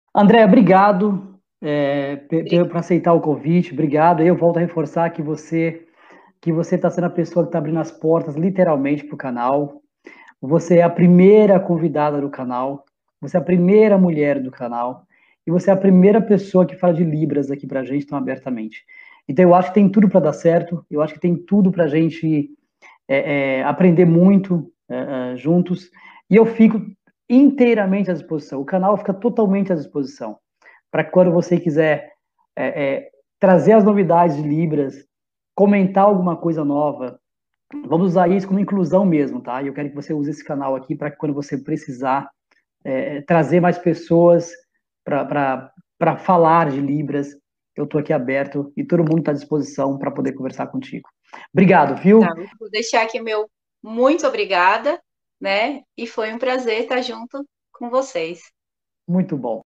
Relembre na Galley - Ândrea Winck - A importância de Libras na aviação.
Vamos relembrar nesse vídeo, nossa primeira convidada do Conversa na Galley, Ândrea Winck, onde exploramos o assunto de libras na aviação.